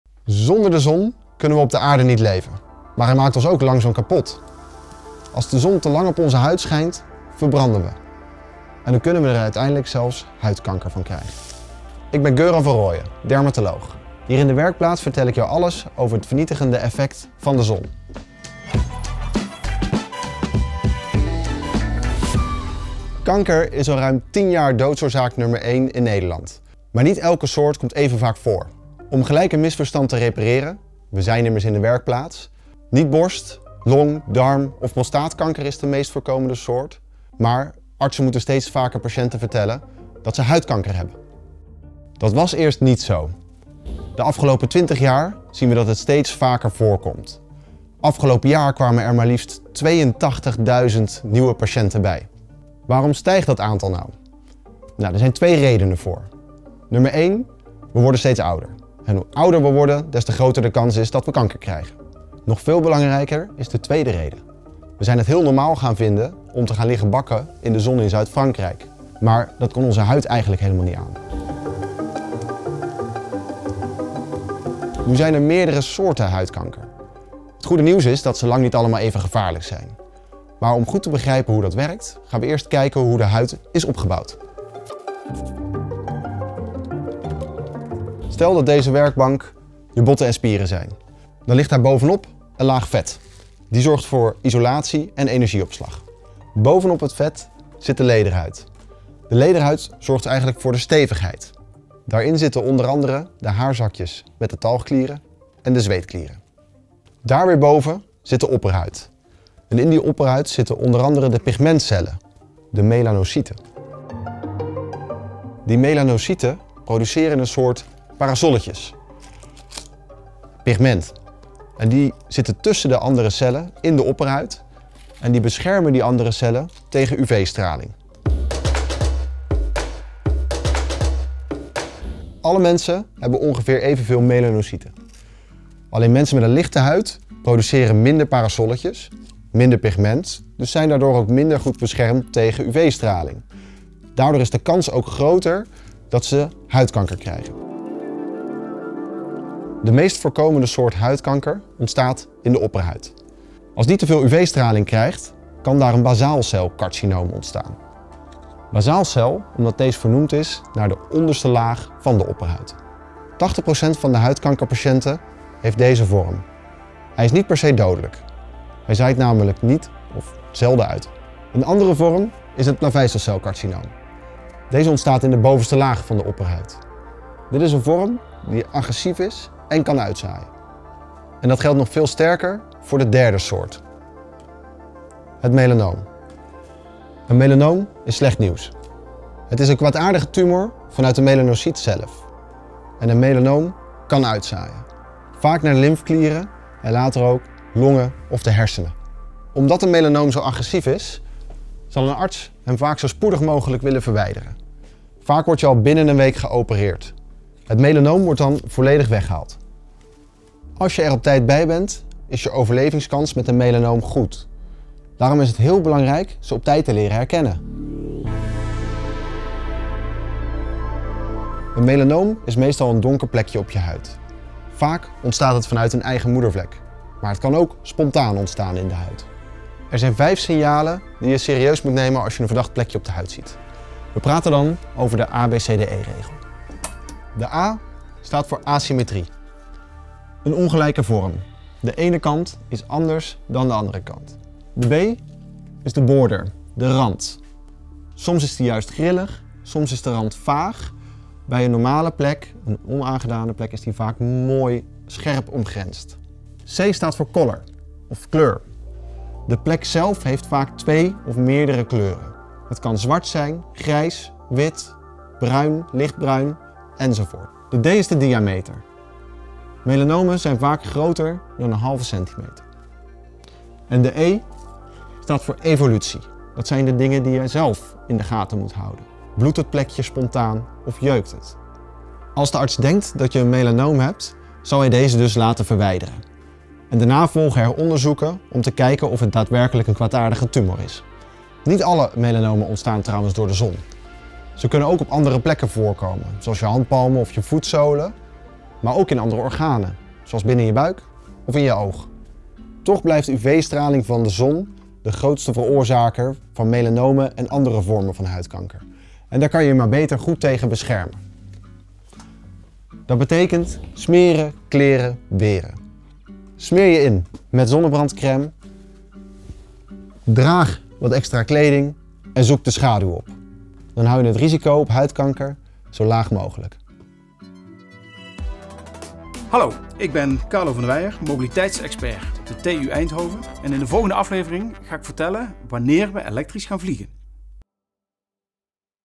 Hoe herken je huidkanker?
De hete zomers van tegenwoordig beginnen hun tol te eisen: huidkanker is in Nederland de laatste dertig jaar uitgegroeid tot de meest voorkomende kankersoort. Afgelopen jaar moesten artsen maar liefst 82.000 keer aan patiënten vertellen dat in hun huid een of meerdere tumoren waren aangetroffen. Eigen schuld? Zeker niet altijd, maar we kunnen ons risico op huidkanker wel degelijk verkleinen. Zo legt dermatoloog Göran van Rooijen (Erasmus MC) in deze aflevering van de Werkplaats uit waar je op kan letten.

0:00 Het vernietigende effect van de zon
0:23 Welke kankers komen het vaakst voor?
0:52 Waarom stijgt het aantal huidkankerpatiënten?
1:27 Deze mensen krijgen het vaakst huidkanker
2:30 De meest voorkomende kankers in de huid
3:01 Een melanoom: slecht nieuws
3:41 Hoe herken je huidkanker?
5:03 Hoe voorkom je huidkanker?

En wil je meer weten over zonnebrandcrème? Kijk dan deze video waarin scheikundige Heleen Kibbelaar (Universiteit van Amsterdam) alles uitlegt over zonnebrandcrème.